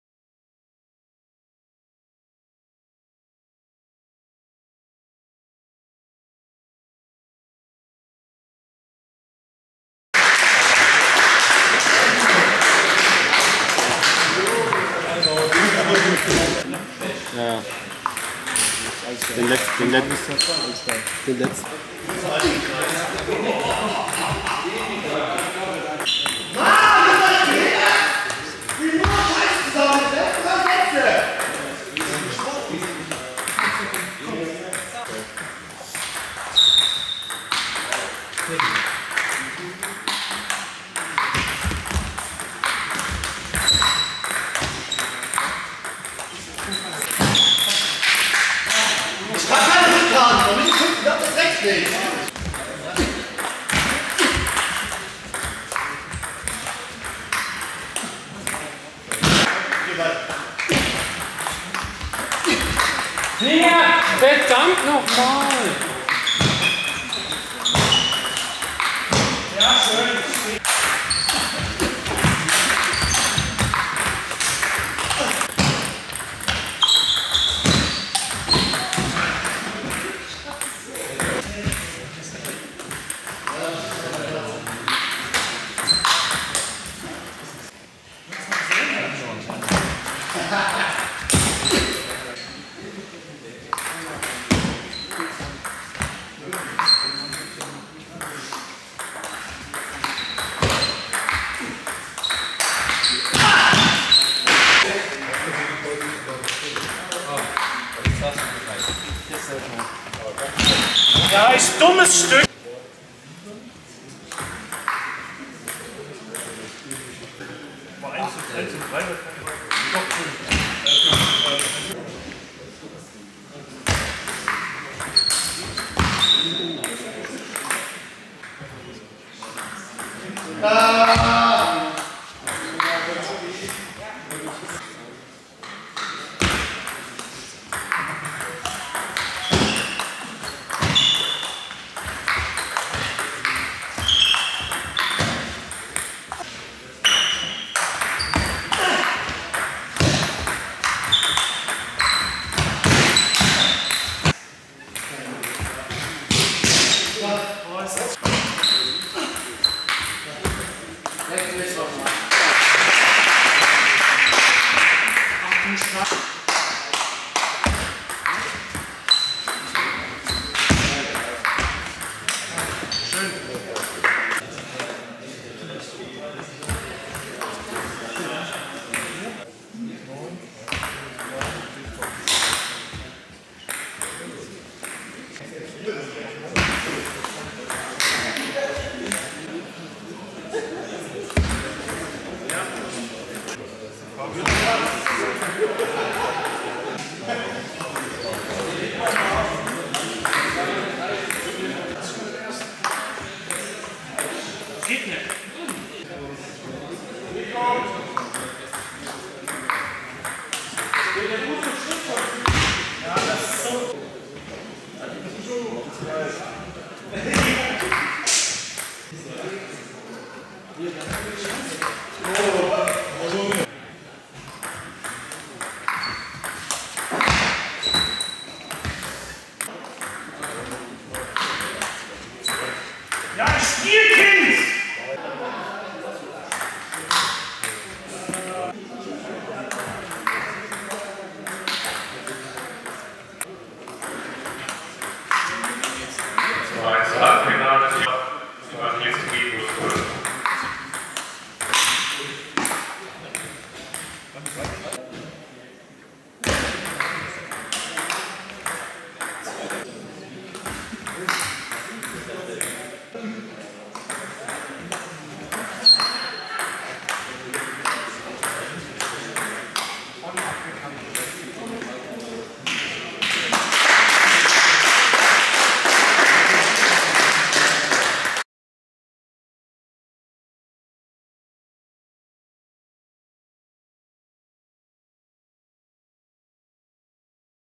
Vorgabeturnier Niederelbert Halbfinale Otterbach.K : Döll.A
Die Nummer 1 und 3 gesetzten  Kai Otterbach & Alexander Döll Spielen um den Final Einzug